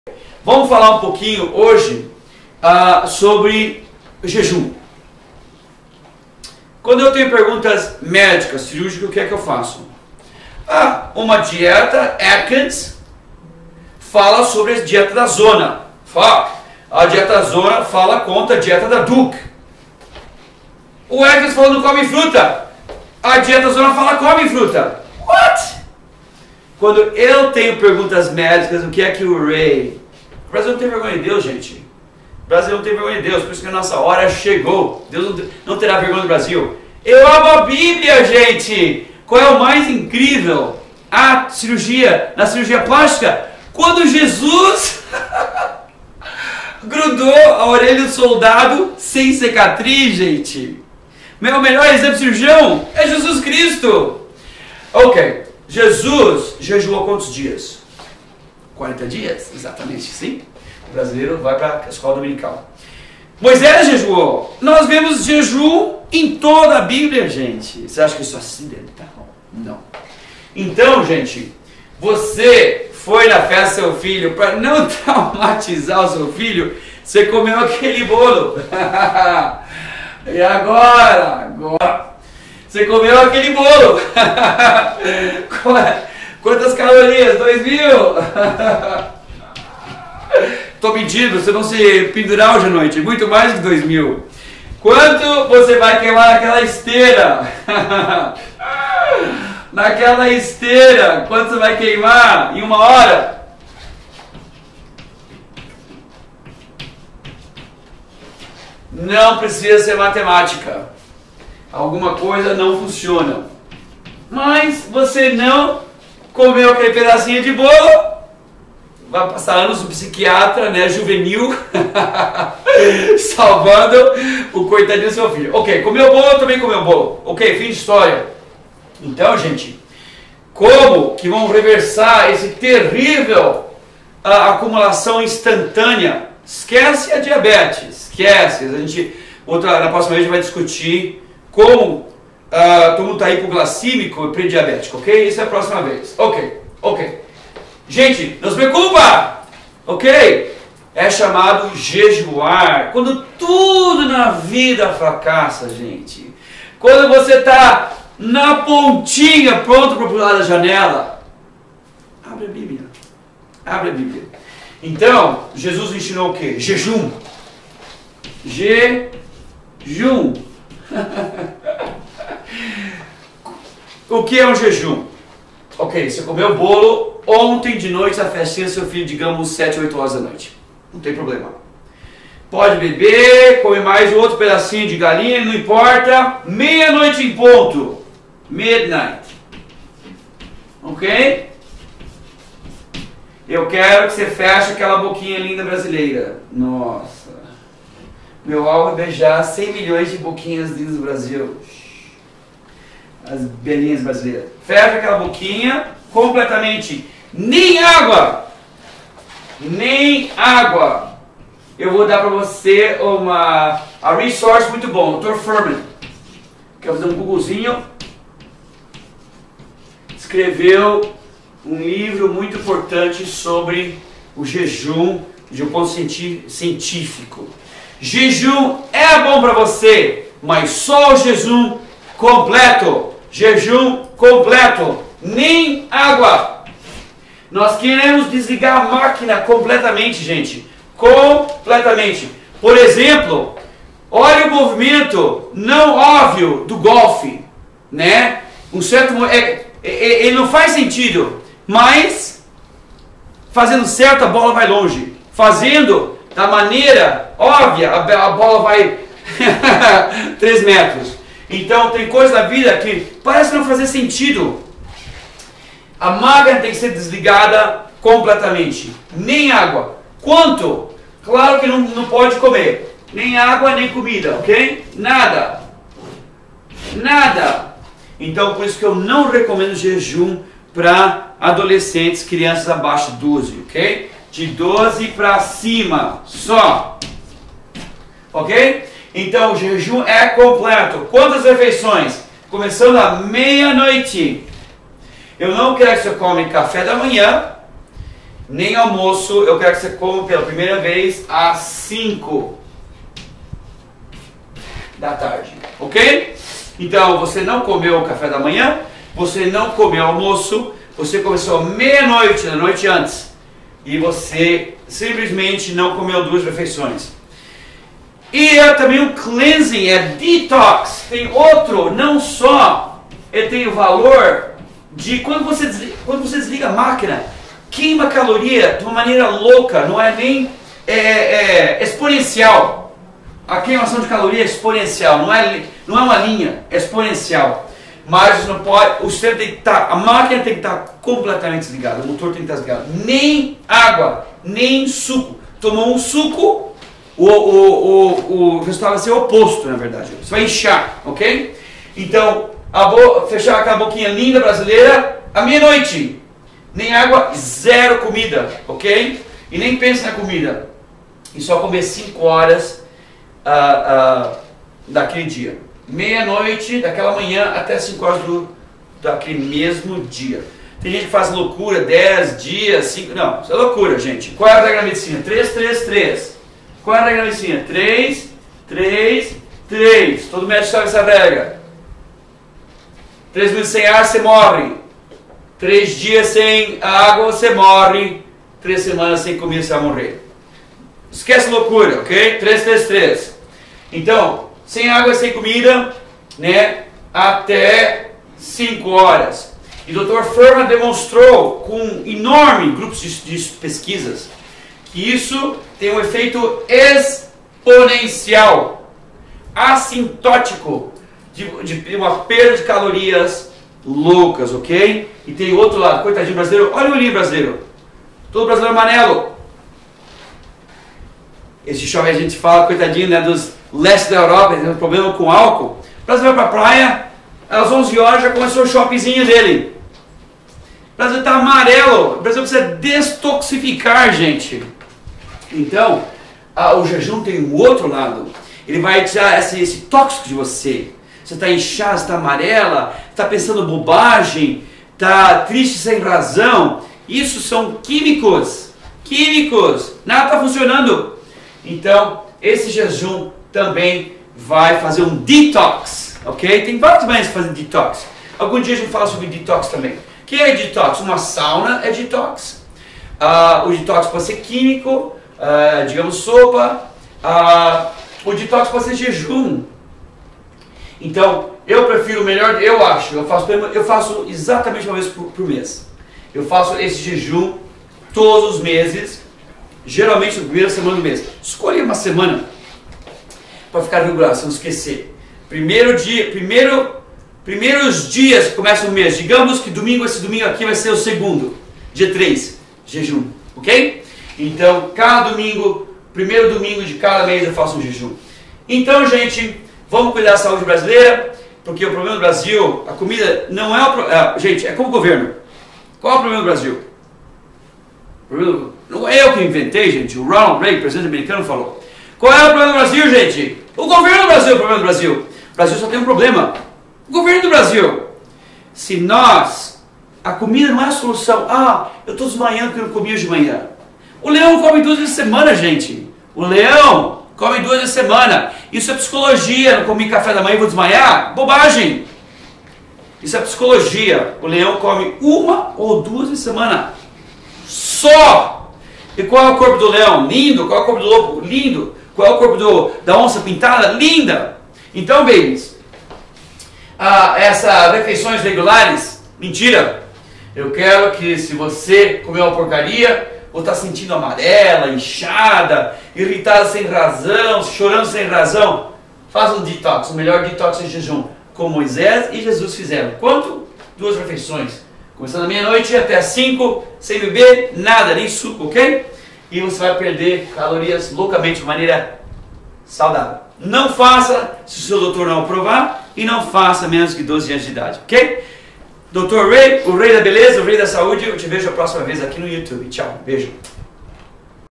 Dr. Rey - Jejum - perca peso e resolva o problema quando comer demais!
👉LIVRO "DIETA DR. REY" - PROMOÇÃO POR TEMPO LIMITADO!!! SOMENTE NO SITE DA PRATA EDITORA! 🇧🇷🇧🇷❤📖📖 👉CLIQUE NO LINK PARA SABER MAIS E COMPRAR

O jejum é uma poderosa arma contra o ganho de peso. Você pode fazer com regularidade ou para compensar as vezes que você "exagera" na comida!! Eu vou te explicar como funciona e como fazer para usar o jejum para se manter em forma!